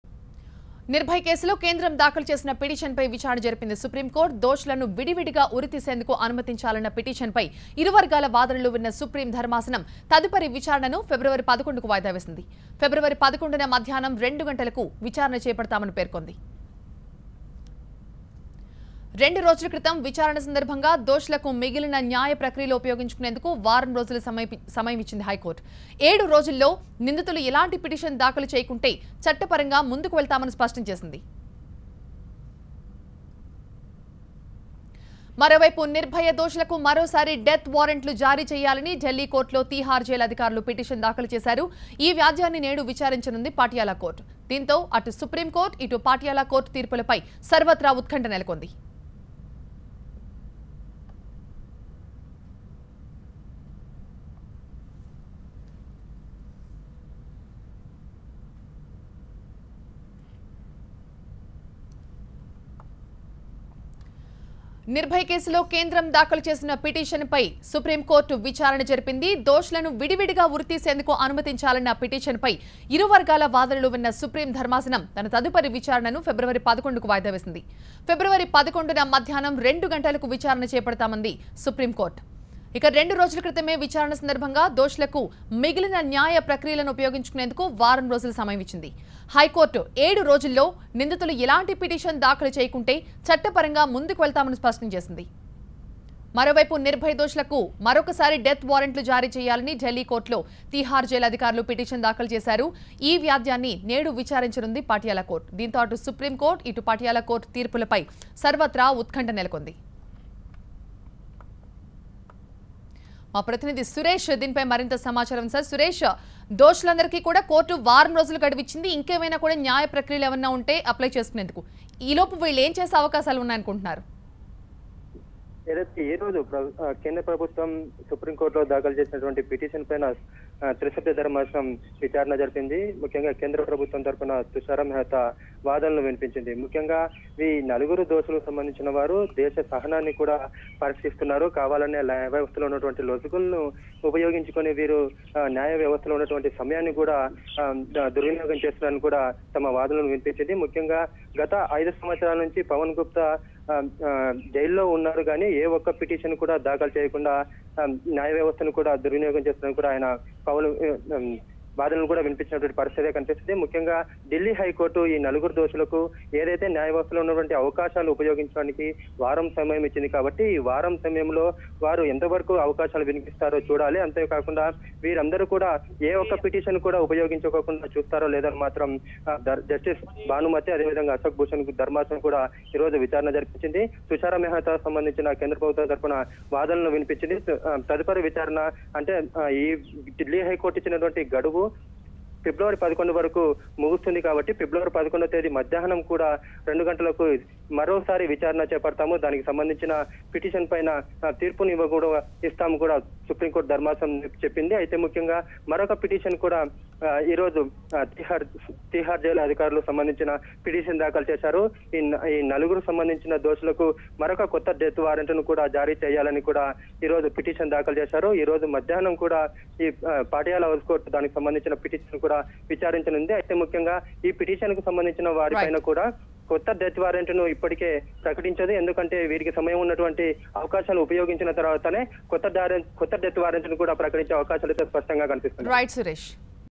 Nirbhaya case: SC to hear February 11 Centre’s plea against Delhi HC verdict - TV9
Nirbhaya case: SC to hear February 11 Centre’s plea against Delhi HC verdict